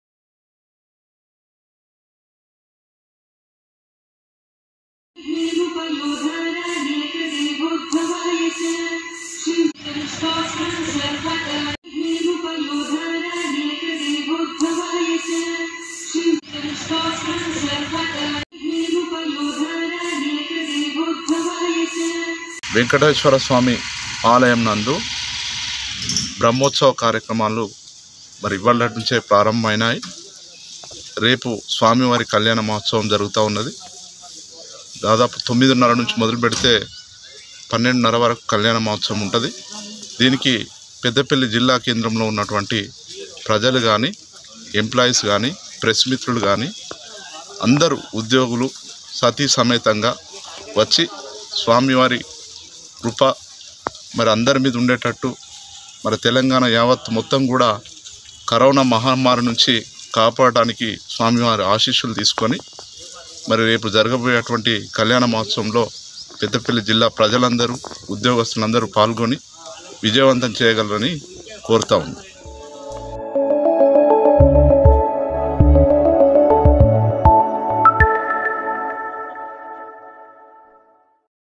శ్రీ వెంకటేశ్వర స్వామి బ్రహ్మోత్సవాలకు భక్తులు అధిక సంఖ్య లో పాల్గొనాలి: బొంకూరి శంకర్ ,చ్తెర్మన్
Thank you for your support to "Gemini Bharat News" channel🙏